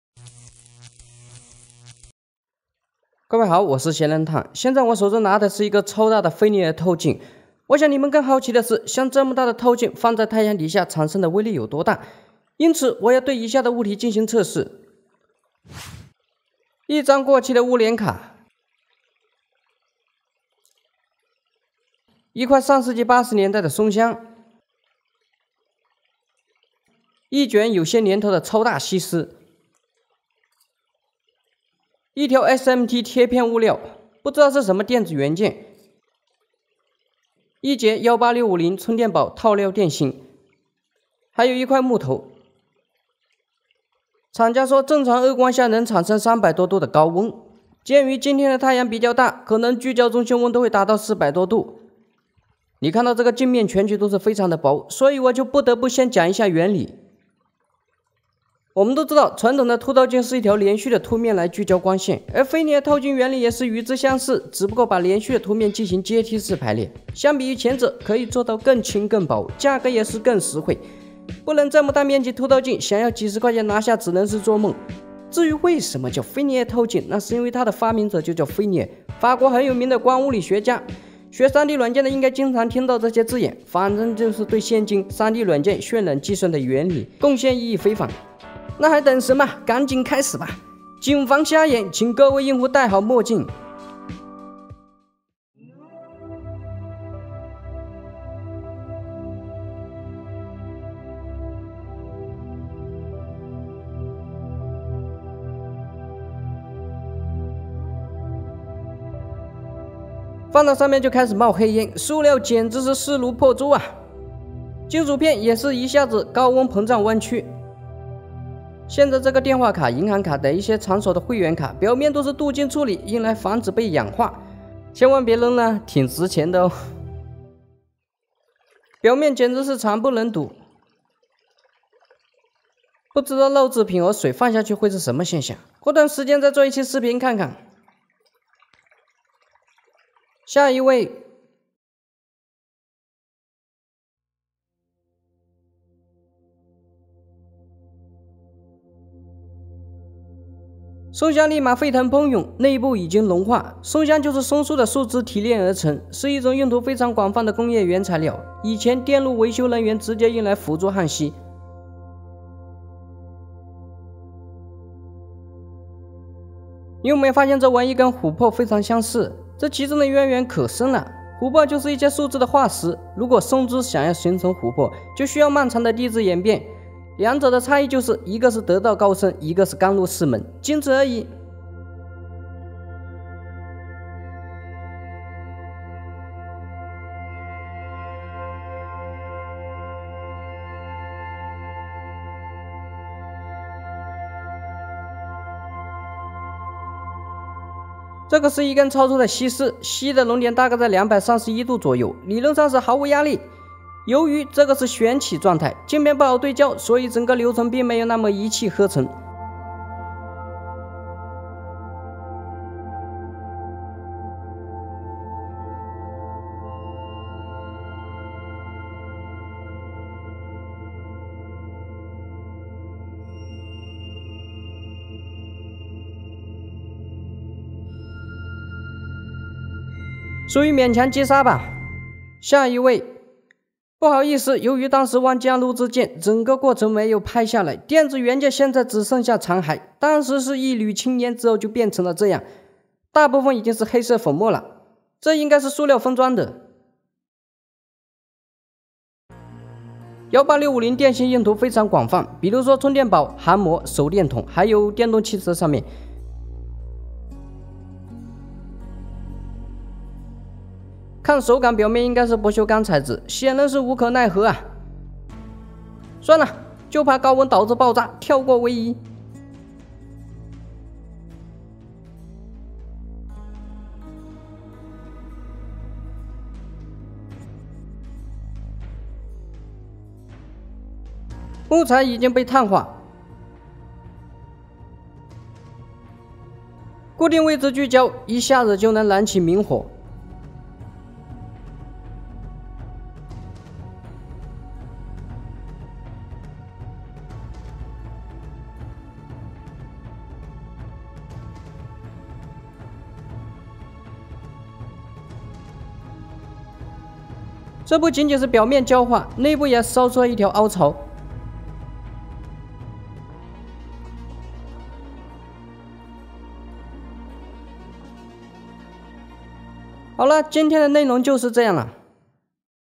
把這樣的放大鏡，放在太陽下聚焦溫度會有多高呢？
暂无说明，看视频吧！